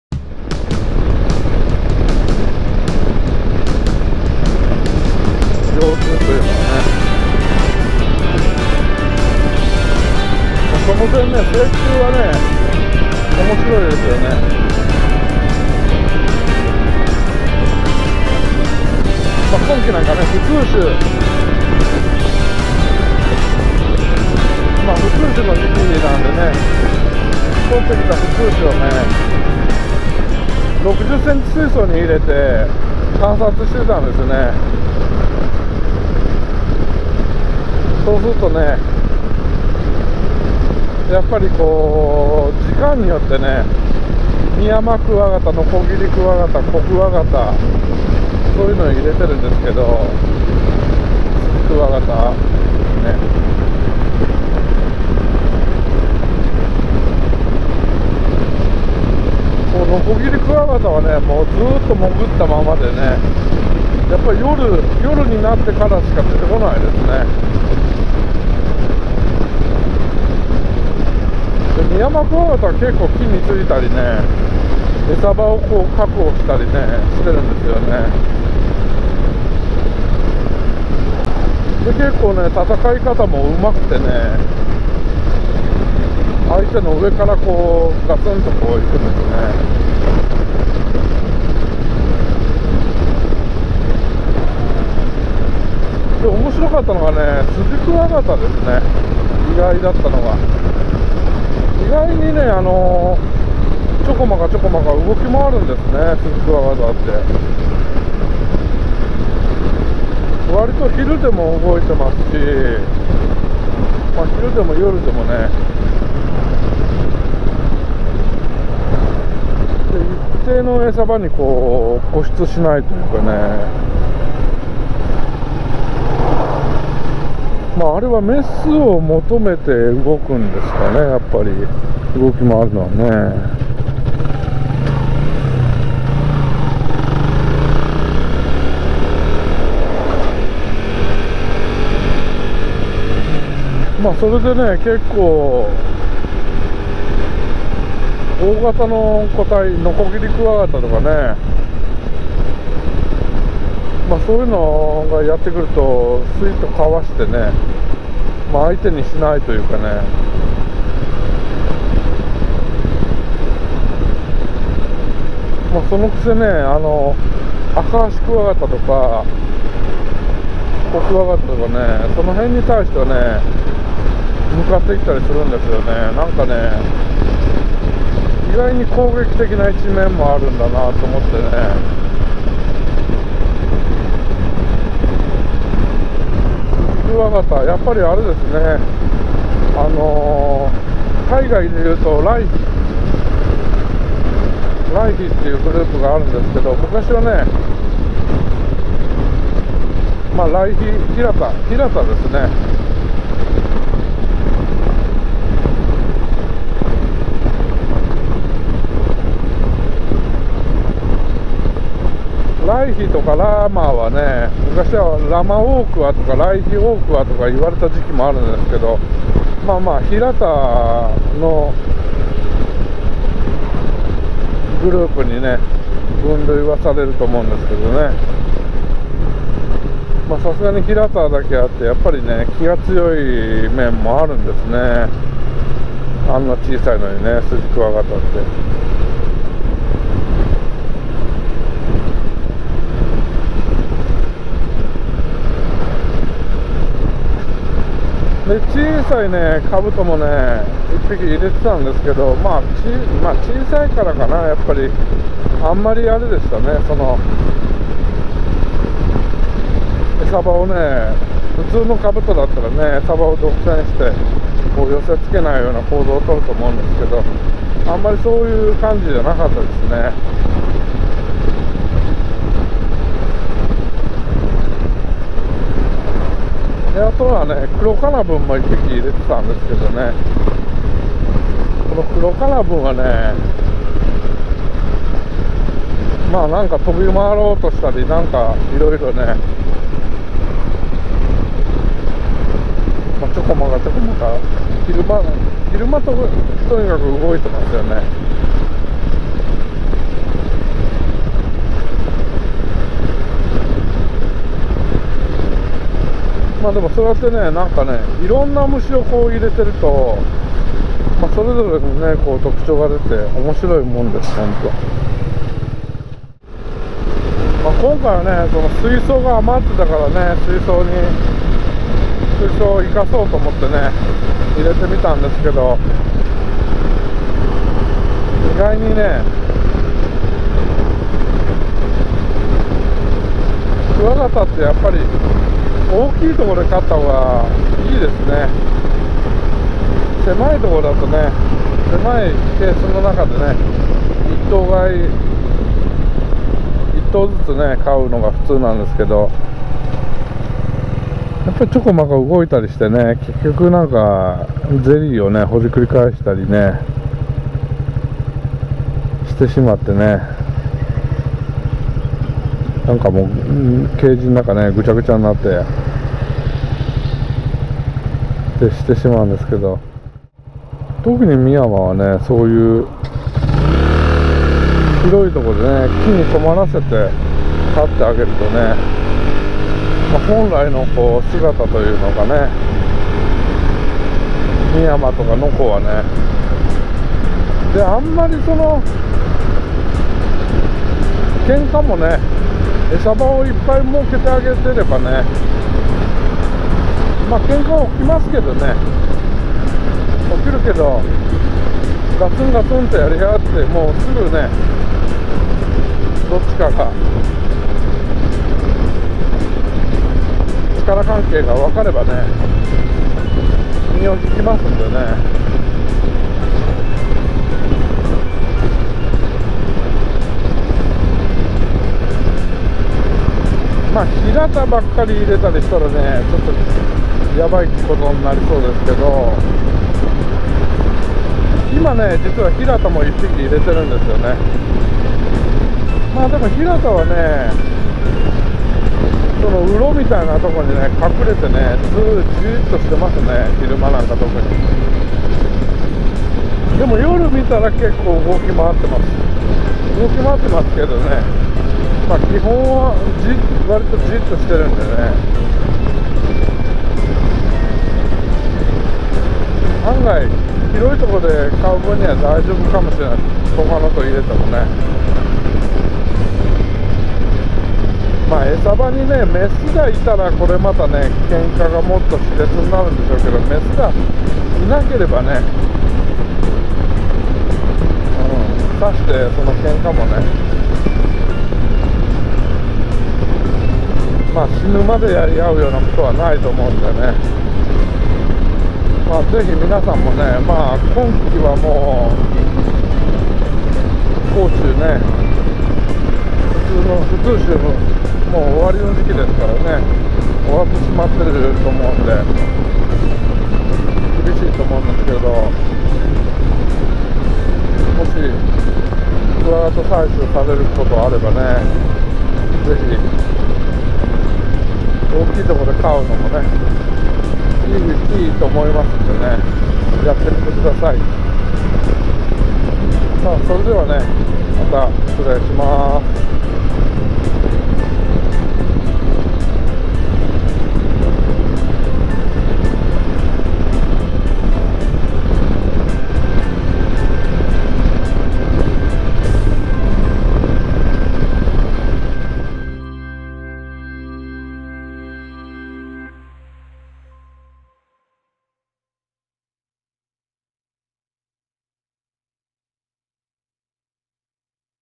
【クワガタ観察】クワガタテラリウム 昆虫観察について masa'skuwavlog
採集で持ち帰った普通種のクワガタを60cm水槽に入れ観察していると、時間によって活動する虫も違って面白いもです。

ちなみにテラリウムに入れている昆虫は下記になります。（★となった虫も含みます）
・ミヤマクワガタ ×♂6
・ノコギリクワガタ ×♂6
・ヒラタクワガタ ×♂1
・コクワガタ ×♂2
・スジクワガタ ×♂3
・アカアシクワガタ ×♂1
・カブトムシ ×♂1
・クロカナブン ×1
・シロテンハナムグリ ×1

使用機材：GoPro HERO8 BLACK、SONY RX-100